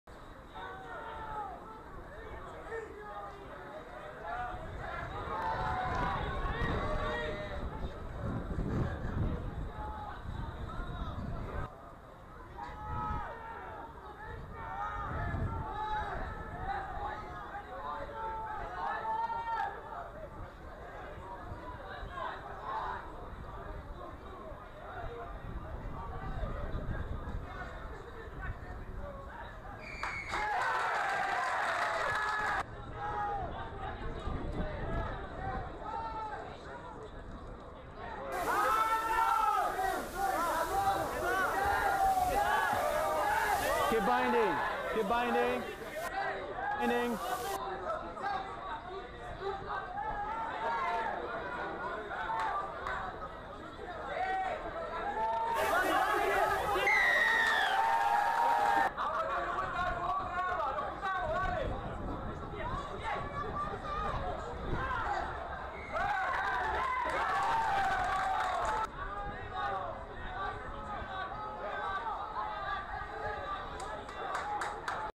Uruguay U20 crushes Canada U20 scrum
Uruguay U20 slowly and steadily crush the Canada U20 scrum with a series of big drives that includes a pushover try during the U20 Trophy match in September 2018 at the Arcul de Triumf in Bucharest.